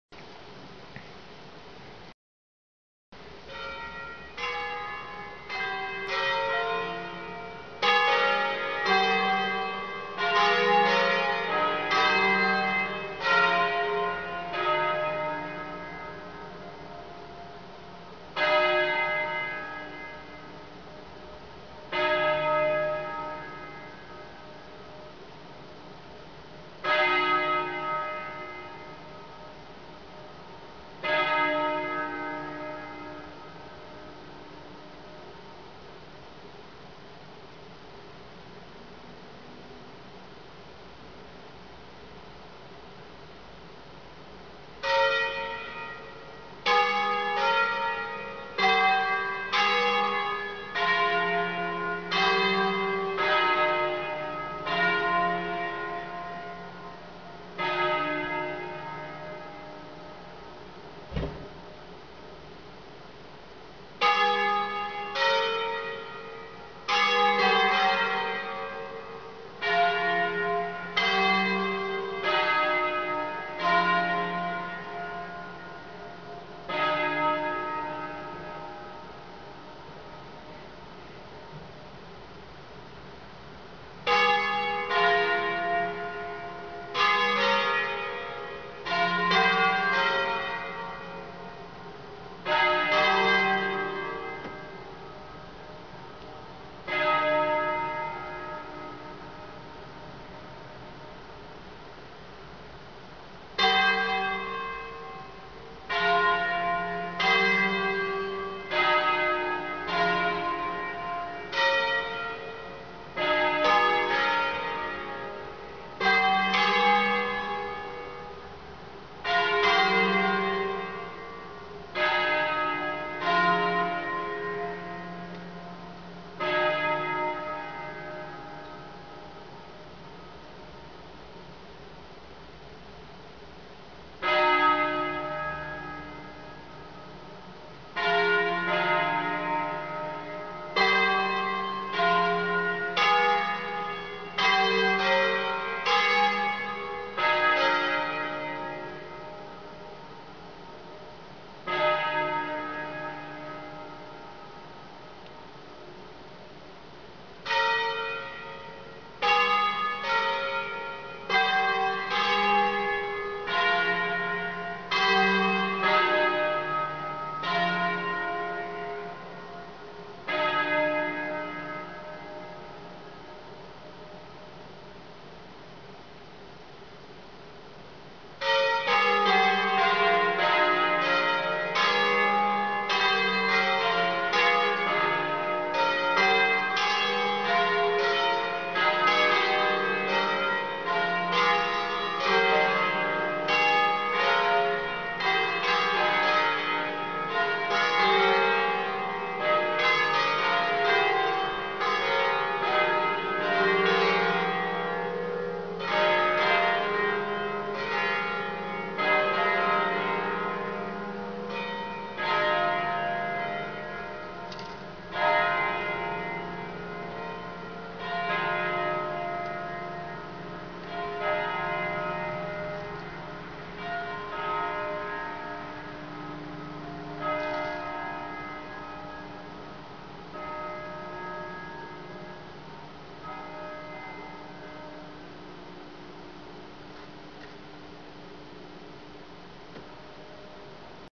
Campane del Santuario della Beata Vergine di Gallivaggio (SO)
Concerto - Concerto ambrosiano di 5 campane, mib3, fa3, sol3, lab3 e sib3 - Martedì 1 novembre 2016, h. 16.15 e domenica 30 ottobre 2016, h. 16.15 - Le due campane più antiche, fa e sol risalgono al 1730 (rispettivamente 100 e 70 pesi) e furono donate dagli emigranti di Roma e Palermo. Ad esse si aggiunsero le due del 1769, donate da emigranti e devoti. ll fa3 (kg. 758) fu rifuso dalla ditta G. Pruneri di Grosio nel 1869, il sol3 fu rifuso dalla ditta G. Pruneri di Grosio nella seconda metà del secolo XIX. Nel secondo dopoguerra tutte e quattro le campane furono rifuse dalla ditta Filippi di Chiari, con l'aggiunta della quinta Cfr. e